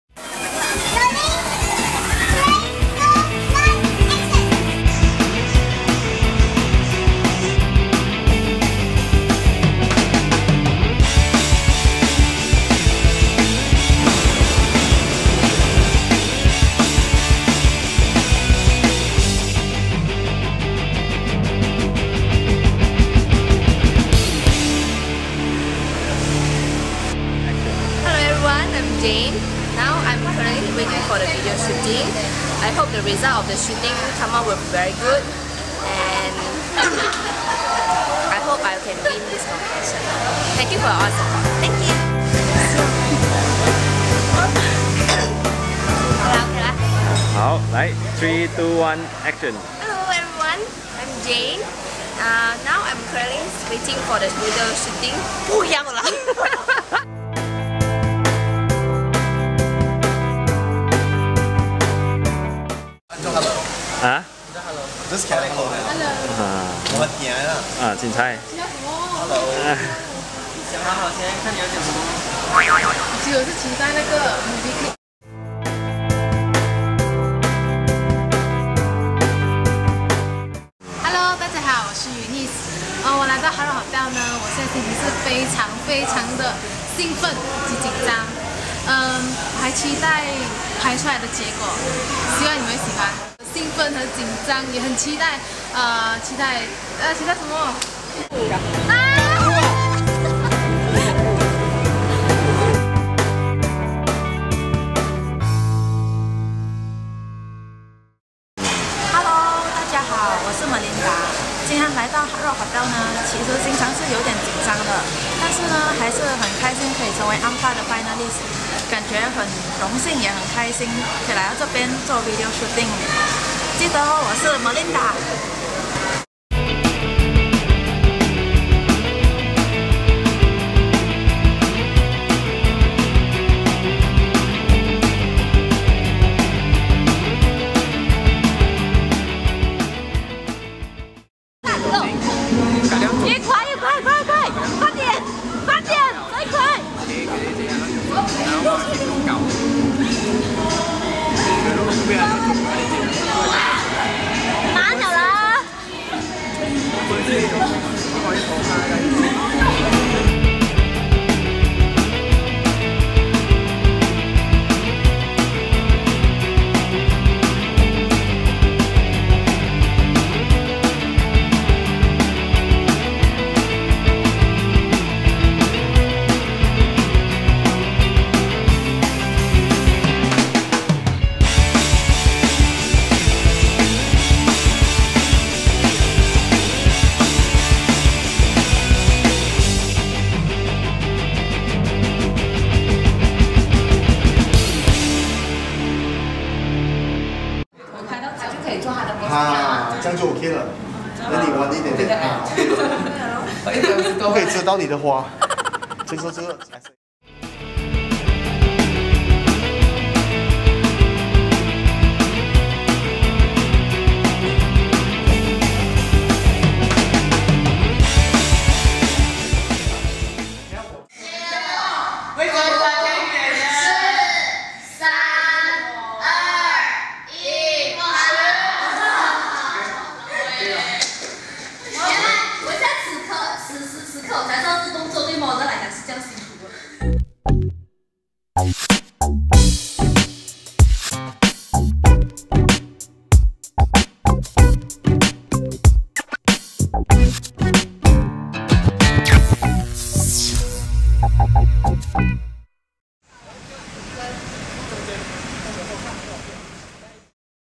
The Making of AMFA MV
Asia Model Festival Finale MV Making

Thank You to the Organiser 'Intrenasionale' and Partner's 'Amber Chia Academy' for giving the Oppotunities to Work with You.